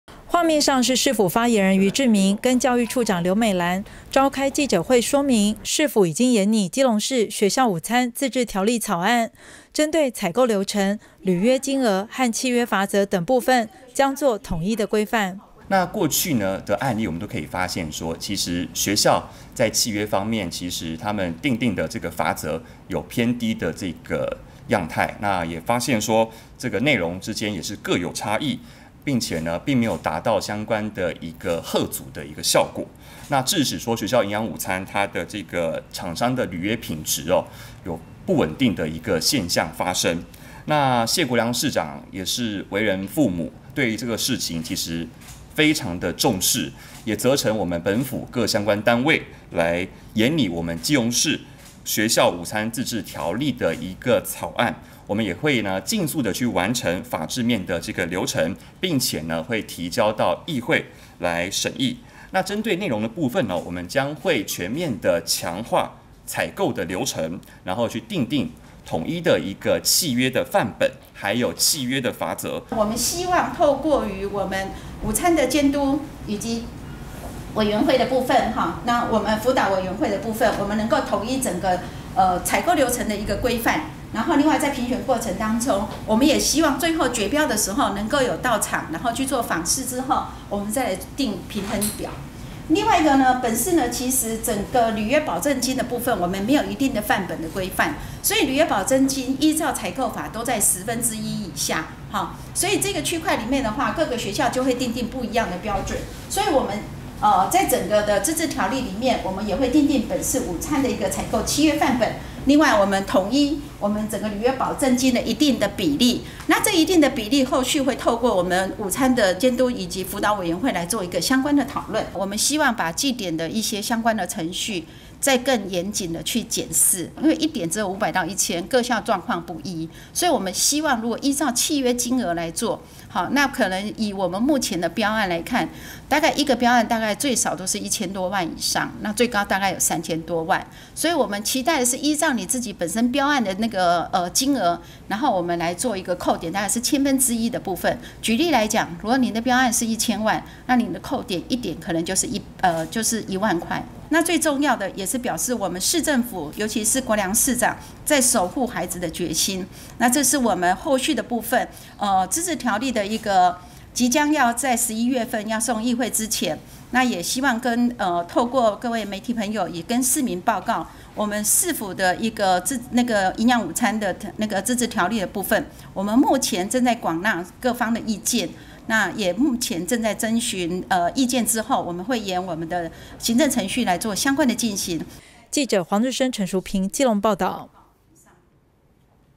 基隆|食安|營養午餐|市府擬定午餐自治條例草案 將送議會審議
為了把關學校營養午餐的品質，基隆市政府召開記者會指出，已經研擬基隆市學校午餐自治條例草案，將盡速完成法制流程，送交議會審議。

鑒於基隆過去發生多起食安疑慮事件，基隆市府發言人余治明、教育處長劉美蘭召開記者會。余治明指出，過去營養午餐案例皆發現契約罰則偏低，未達嚇阻性。為此市府已研擬「基隆市學校午餐自治條例」草案，將成立本市學校午餐食安廉政平台，及學校午餐監督輔導委員會等組織，以地方自治法條的高度和標準，完善基隆學校午餐制度，強化及優化學校午餐廉政流程及品質。

余治明表示，學校營養午餐應照顧孩子的健全發展，除了依約完成契約要求品質外，也應善盡企業社會責任，而過去基隆各校契約罰則偏低未有嚇阻性，致使學校午餐廠商履約品質仍有極大不確定性。謝國樑市長對此事相當重視，並責成市府各相關單位研擬「基隆市學校午餐自治條例草案」，全面強化採購流程、訂定契約範本、契約罰則，並將盡速完成法制流程，交送議會審議。

教育處表示，自治條例除劃分本府各相關單位權責外，將自治條例納入本府政風處，並成立本市學校午餐食安廉政平台，及學校午餐監督輔導委員會，統一規範本府採購流程、契約範本、契約罰則等，擬提高契約罰則記點罰鍰為採購金額千分之一，將履約金額不一致的地方，做統一相關規範，強化午餐食安廉政平台功能。午餐採購資訊及過程由政風單位參與到場監標，並建議學校辦理午餐採購，於評選過程中錄音或錄影，並將評選評分表一併密封留存備查，更請廉政署及檢調單位，定期對學校採購人員進行廉政教育訓練。重視採購公正及公平性，並納入健康飲食及食農教育，型塑健康生活。

教育處長劉美蘭指出，廉政條例辦理過程中除了依照相關流程，與以往不同的是，在訂定自治條例以前，是沒有廉政單位來把關午餐品質，除了成立本次午餐廉政專屬平台，也會請警察單位及檢調單位，針對相關人員進行培訓。其次，在採購過程中也發現採購流程規範其實是交給學校，不見得有一致性，希望透過監督達到統一採購流程規範。

教育處長劉美蘭強調，市府除了強化契約罰則促使食材供應商能把關販售的食材品質之外，也重視學校午餐採購流程的公正及公平性，強化廉政功能及採購流程，市府也透過學校午餐監督輔導委員會訂定基隆的契約範本，若違反相關契約也明訂相關罰則，金額部分會訂定在約千分之一，以底標一千萬來說，千萬分之一罰金為一萬元。

而學校午餐自治條例將在11月提交會議，其內容包含餐食教育，將納入健康飲食及食農教育，妥善運用午餐時間或課餘時間形塑健康生活教育。